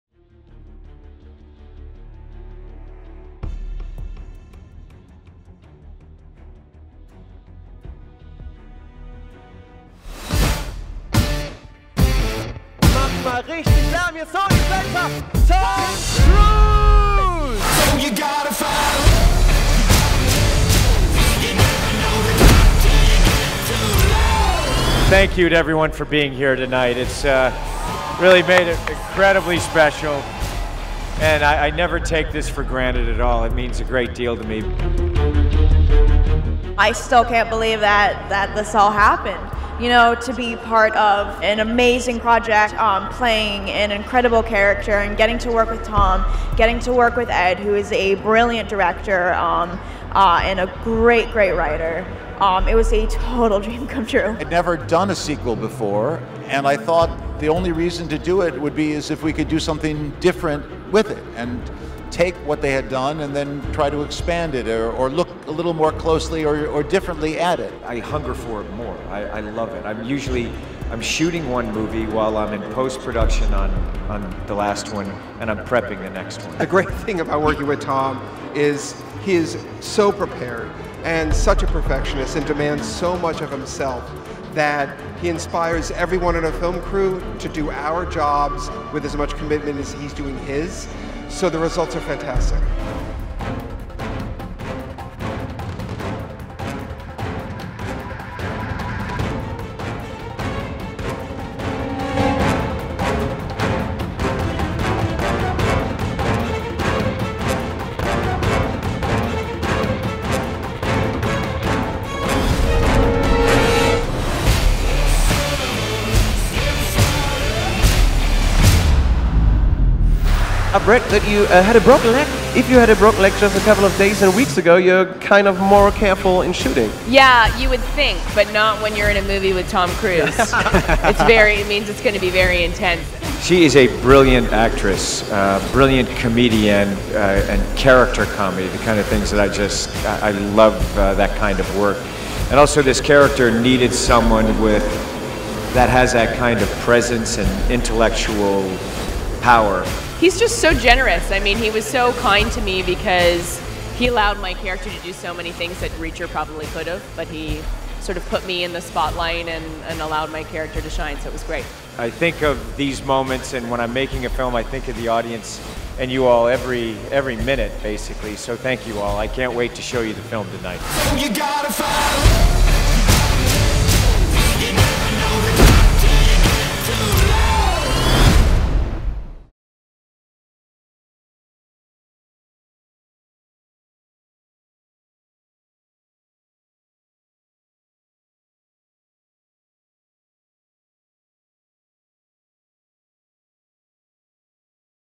JACK REACHER 2: KEIN WEG ZURÜCK | Premiere: 360 Grad Virtual Reality | Im Kino!
Deutschlandpremiere von Jack Reacher 2 in 360 Grad. Tauche ein in die virtuelle Realität und geh neben Tom Cruise, Cobie Smulders und Danika Yarosh, Edward Zwick und Don Granger über den roten Teppich im SonyCenter.
In den deutschen Kinos startet Jack Reacher: Kein Weg zurück - am 10. November.